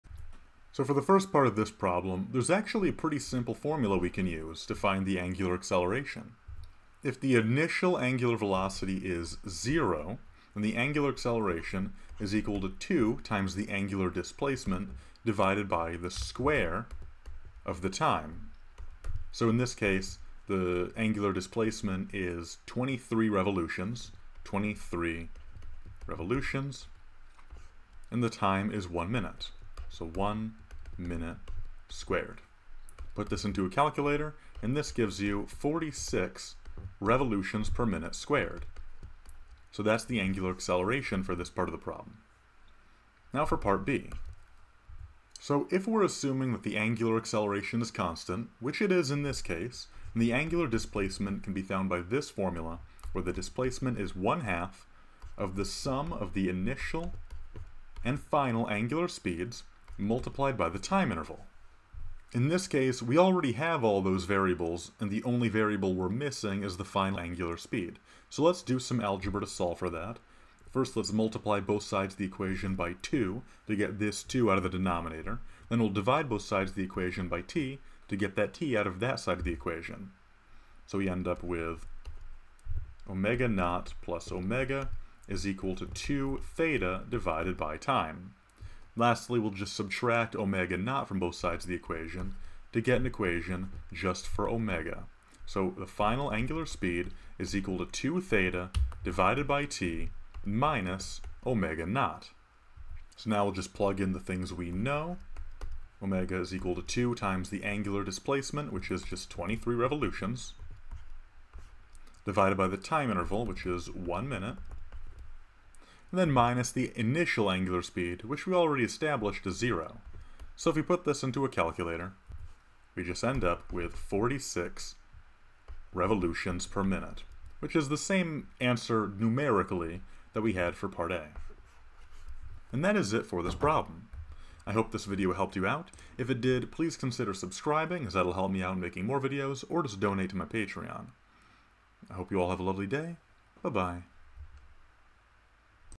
Pilots can be tested for the stresses of flying high-speed jets in a whirling “human centrifuge,”
Pilots can be tested for the stresses of flying high-speed jets in a whirling “human centrifuge,” which takes 1.0 min to turn through 23 complete revolutions before reaching its final speed. (a) What was its angular acceleration (assumed constant), and (b) what was its final angular speed in rpm?